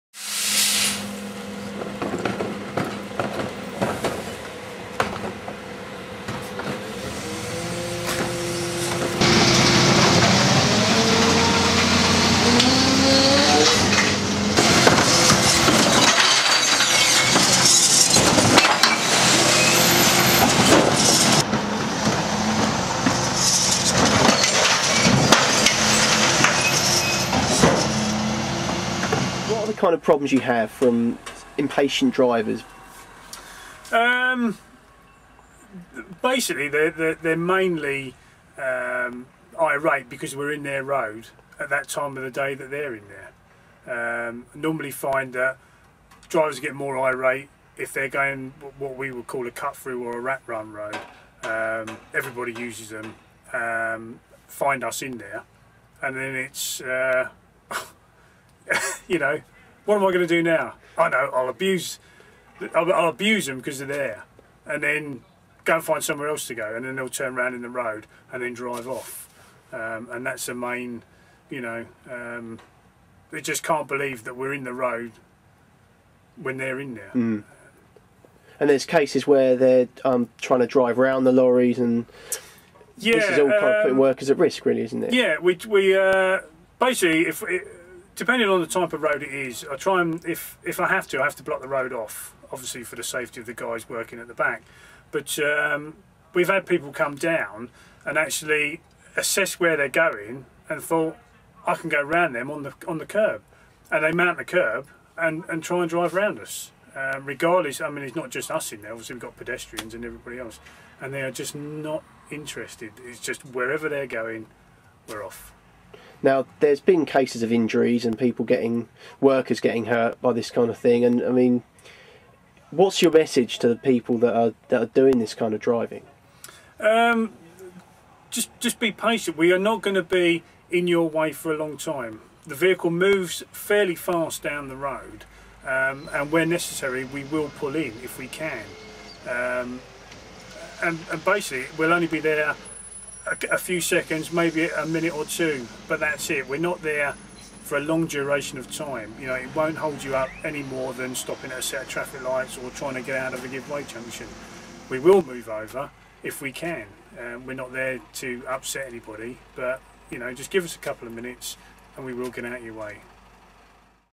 Rubbish collections bin men in danger
Refuse collectors in Worthing and Adur face abuse from motorists as they go about their business.
Herald chief reporter Chris Taylor joined one team on the road while they collected household rubbish, and found out what it is like on the street.